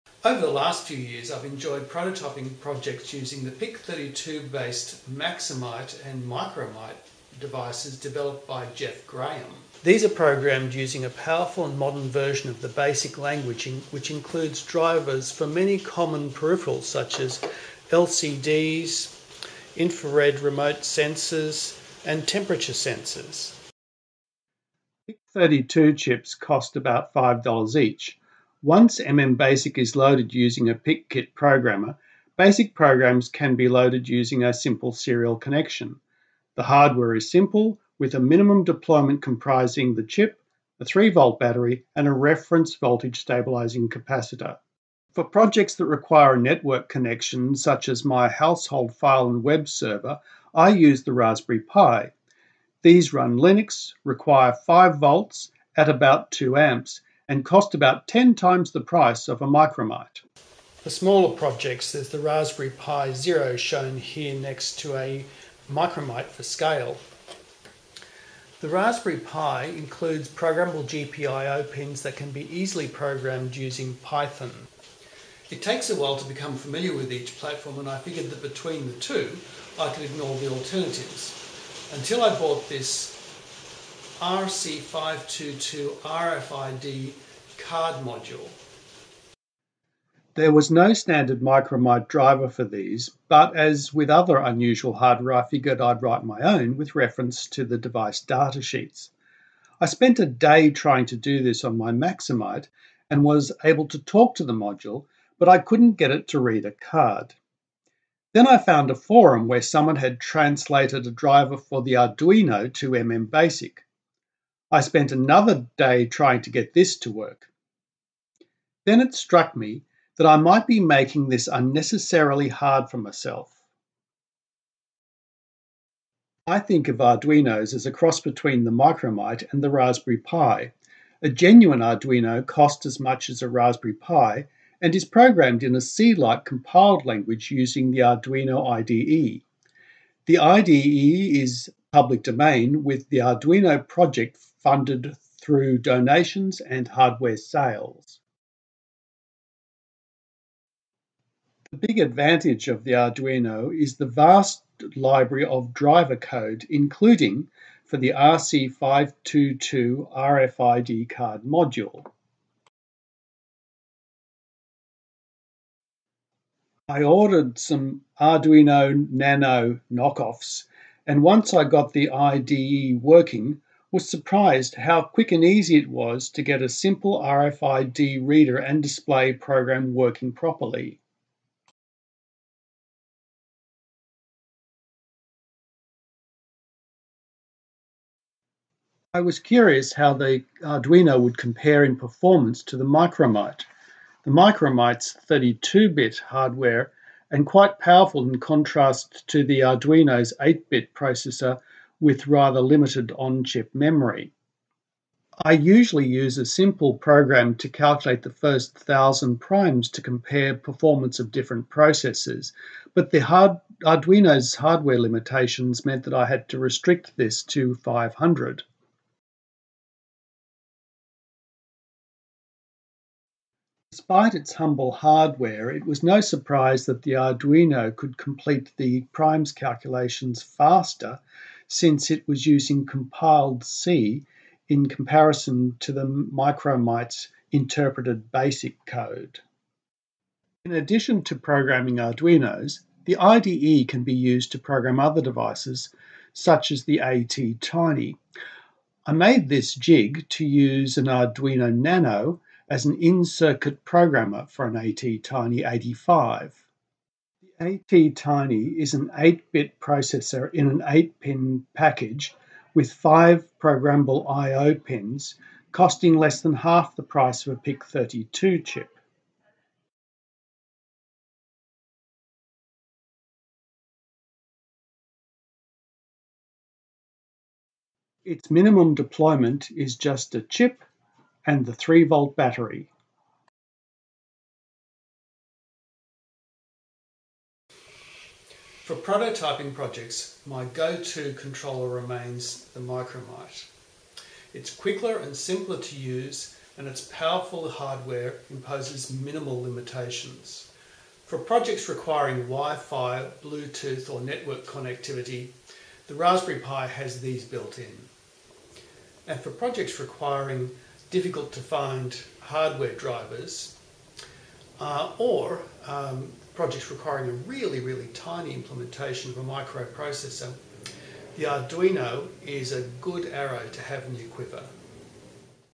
Micromite vs Arduino
Strengths and weaknesses of the Micromite, Arduino and Raspberry Pi.